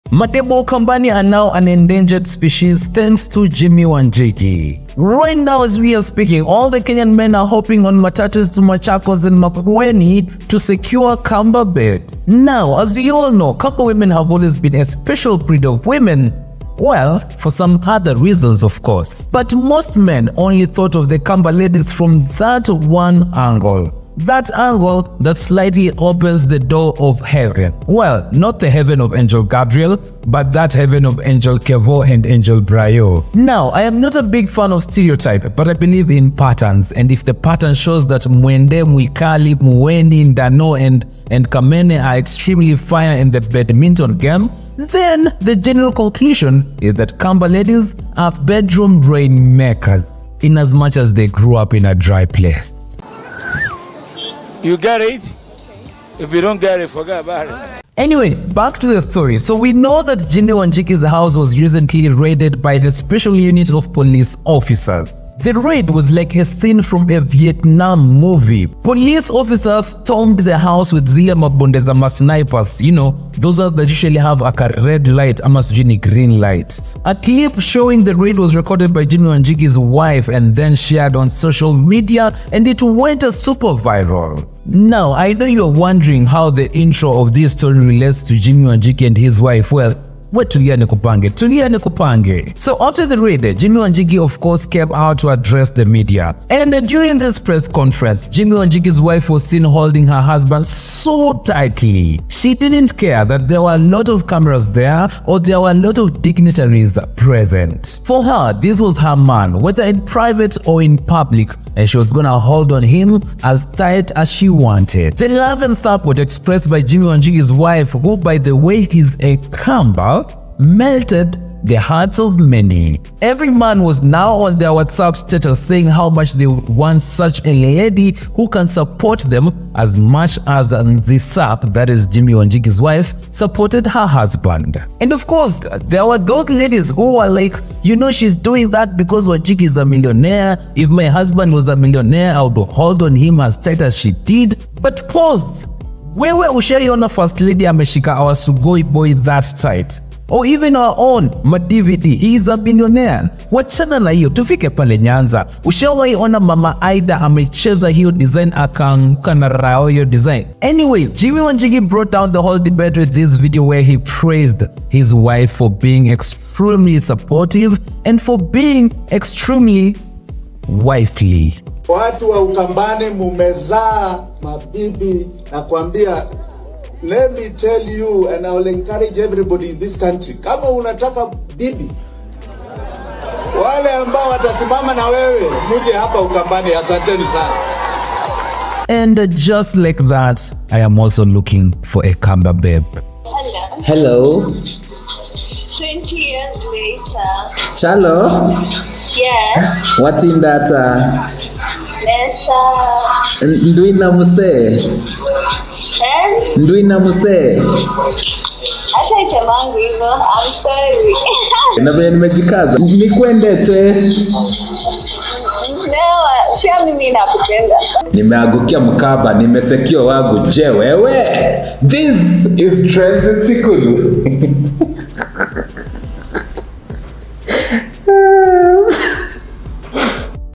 Why Kenyan Men Are Scrambling For Kamba Ladies! What Makes Kamba Ladies Sweet!
Ma Dem Wa Ukambani Are Now an Endangered Species Thanks to Jimi Wanjigi.
Right Now as We Are Speaking All the Kenyan Men Are Hoping on Matatus to Machakos and Makueni to Secure a Kamba Lady.
Watch the full story to find out why.